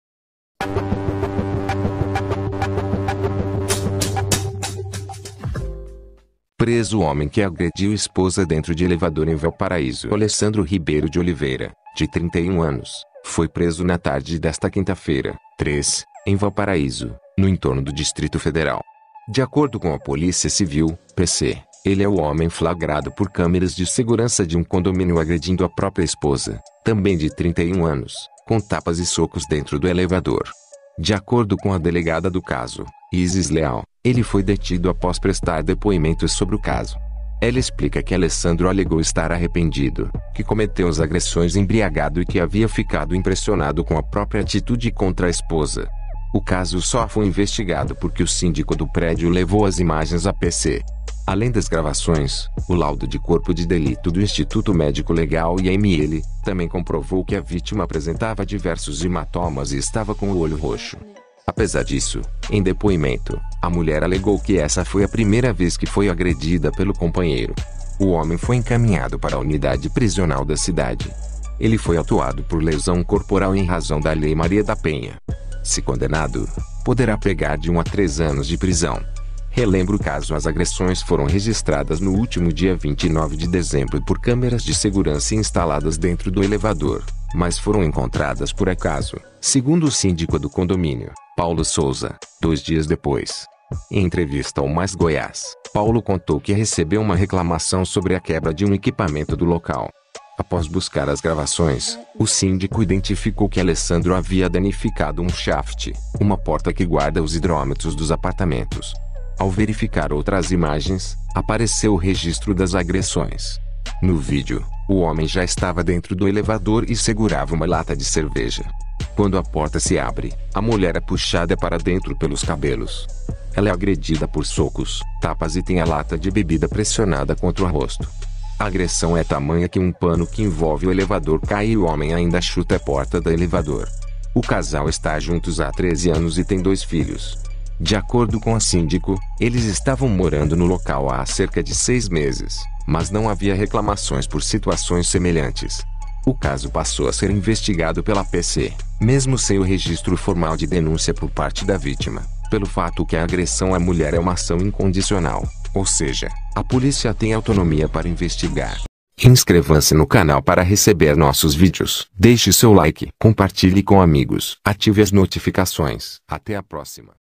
Preso homem que agrediu esposa dentro de elevador em Valparaíso
_*📝Alessandro Ribeiro de Oliveira, de 31 anos, foi preso na tarde desta quinta-feira (3) em Valparaíso, no Entorno do Distrito Federal. De acordo com a Polícia Civil (PC), ele é o homem flagrado por câmeras de segurança de um condomínio agredindo a própria esposa, também de 31 anos, com tapas e socos dentro do elevador.*_